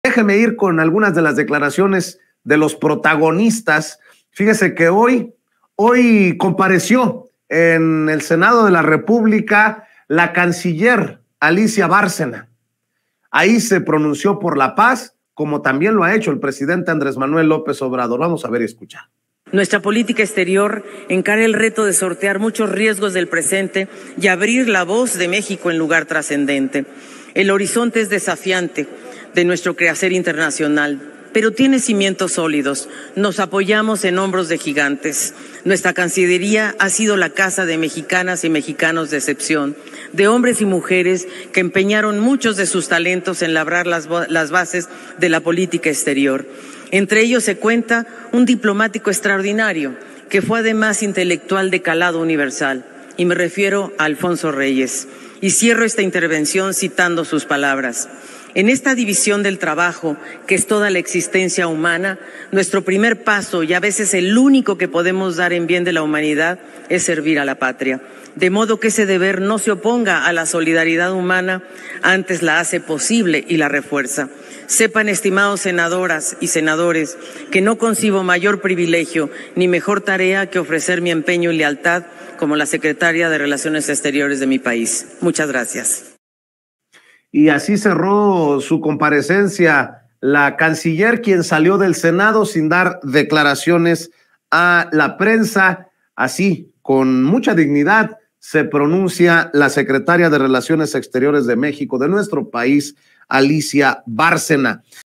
SRE DEFIENDE POLÍT1C4 DE P4Z DE MÉX1C0
Alicia Bárcena, titular de la Secretaría de Relaciones Exteriores, compareció ante el Senado de la República desde donde se pronunció en favor de la paz y elogió la labor de quienes han encabezado la dependencia, entre ellos el escritor Alfonso Reyes